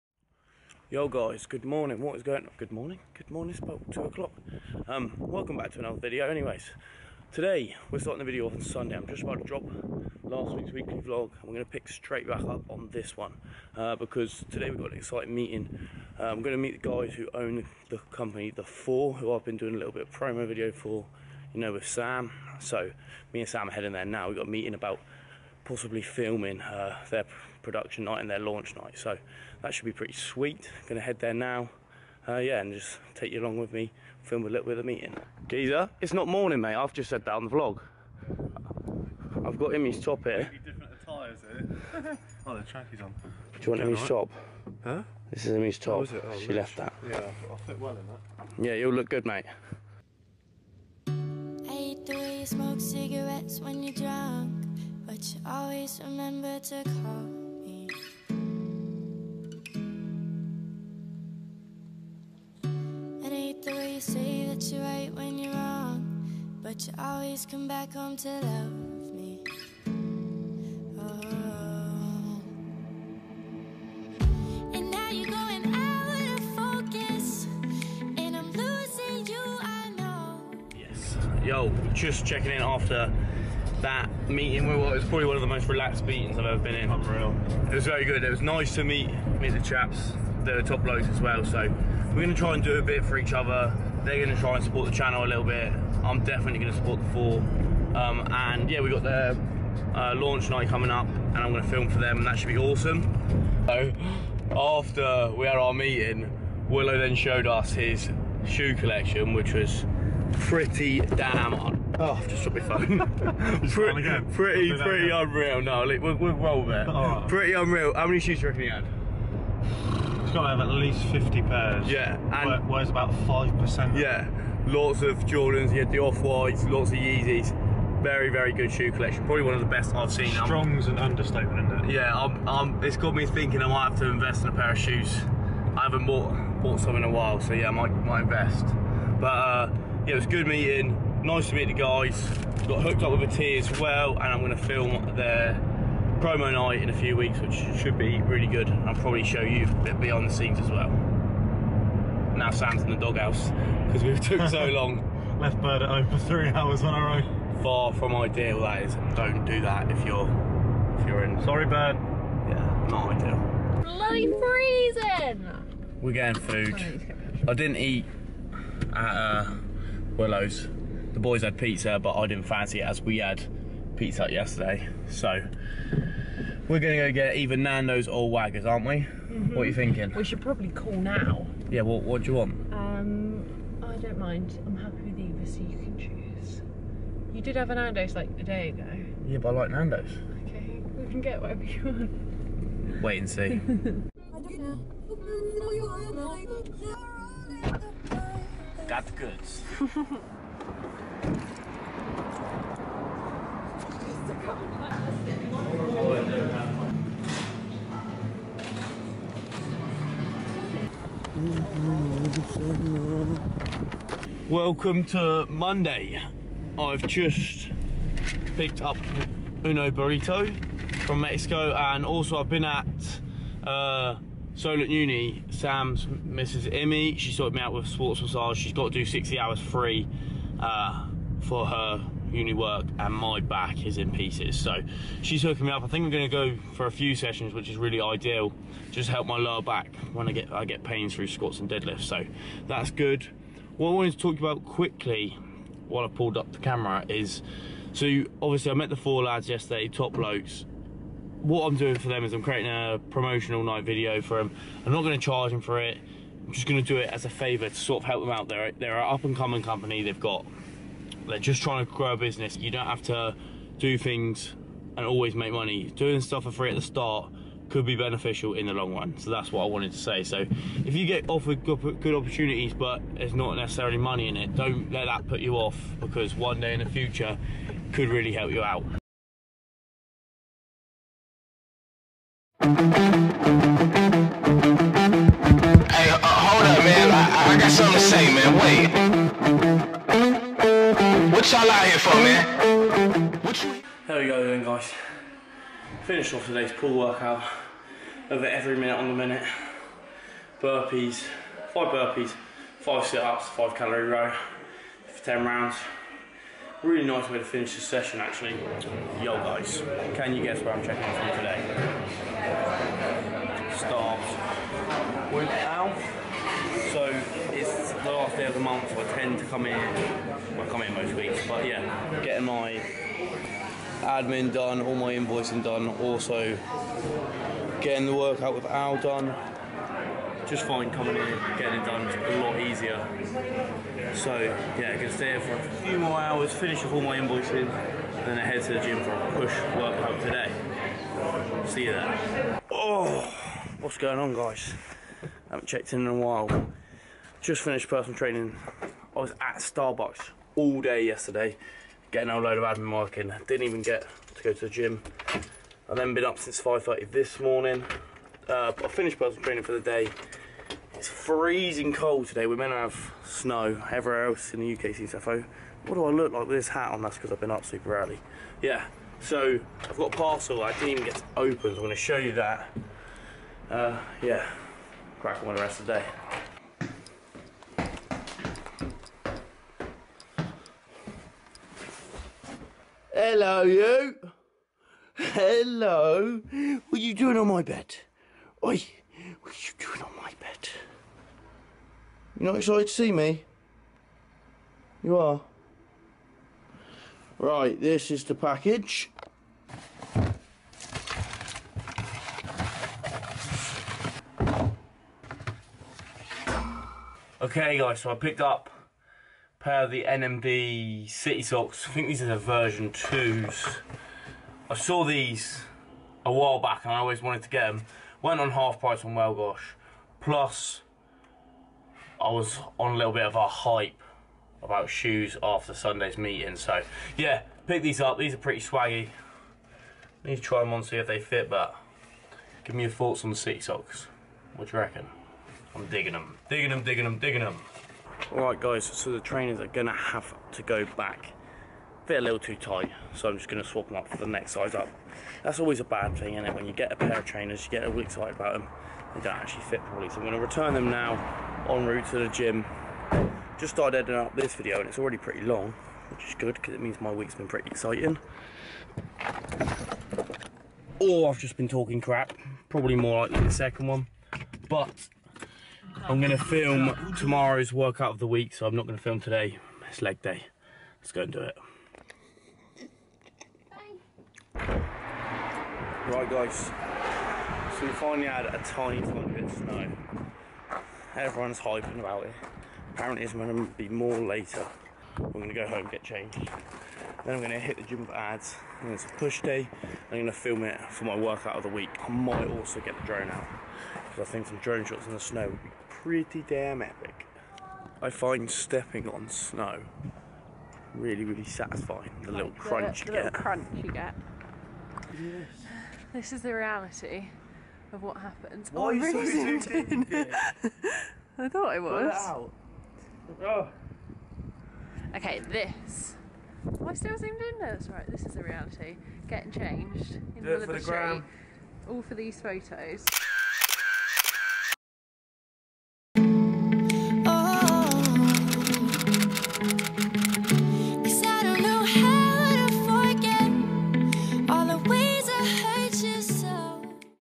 Weekly Vlog 3 - My Trainer Addiction Is Back...
Hi guys, welcome back to this weeks video and the third weekly vlog! This video is full of training, meetings and coffee... so a pretty standard week! I cut this one a bit short so that I can make an extra snow day video with all the snowy weather we have been having.